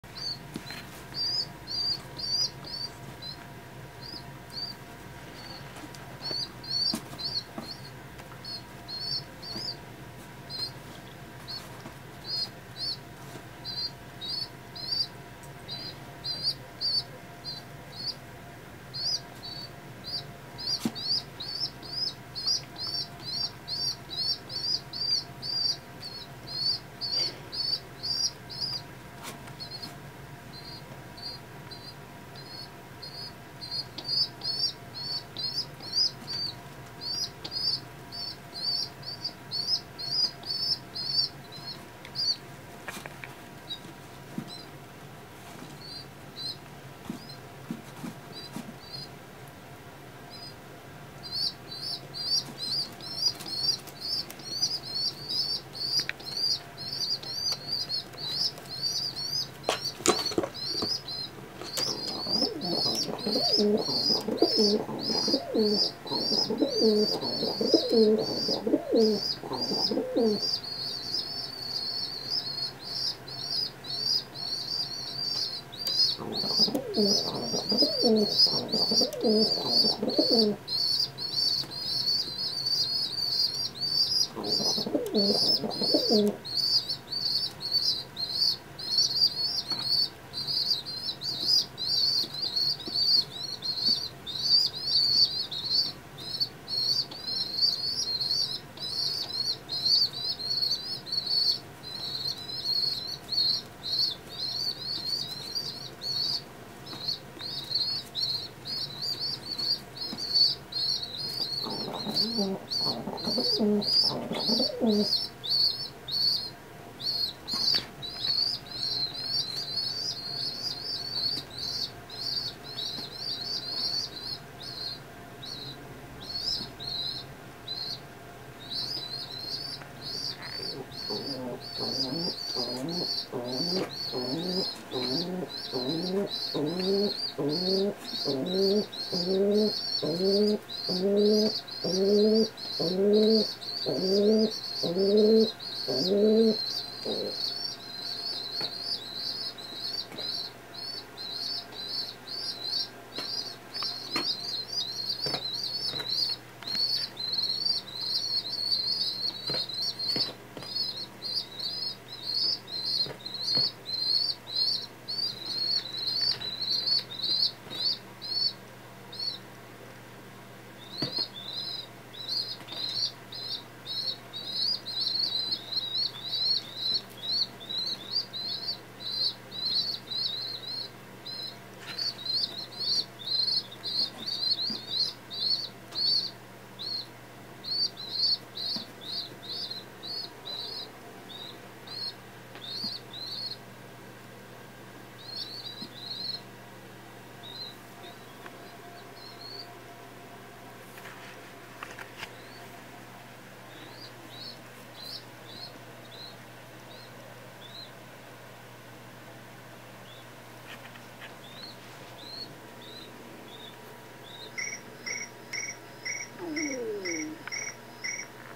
Takeo feeding the babies
Those baby birds are so awkward lol.